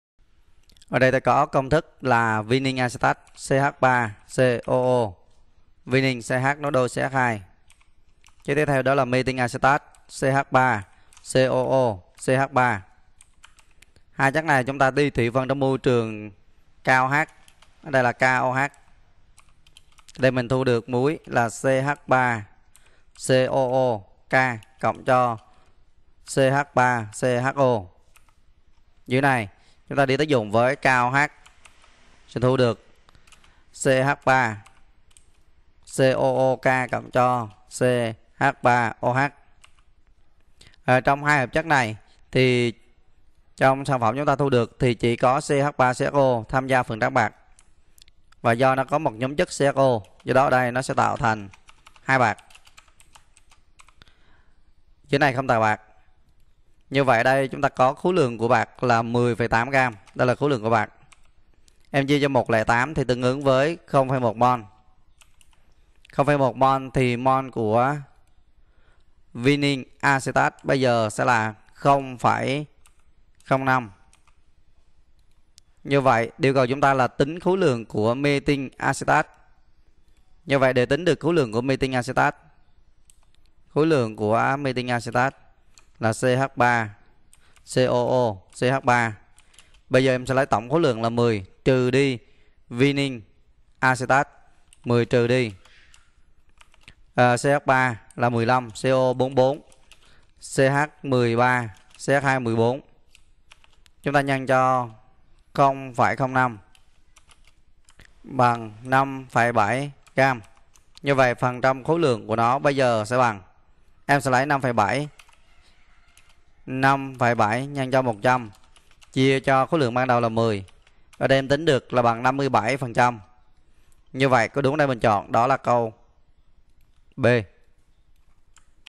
Thủy phân hoàn toàn 10 gam hỗn hợp A gồm vinyl axetat và metyl axetat trong dung dịch KOH. Cho toàn
Thủy phân hoàn toàn 10 gam hỗn hợp A gồm vinyl axetat và metyl axetat trong dung dịch KOH. Cho toàn bộ sản phẩm tác dụng dung dịch AgNO3/ NH3 dư thu được 10,8 gam kết tủa. Phần trăm khối lượng của metyl axetat trong hỗn hợp A là: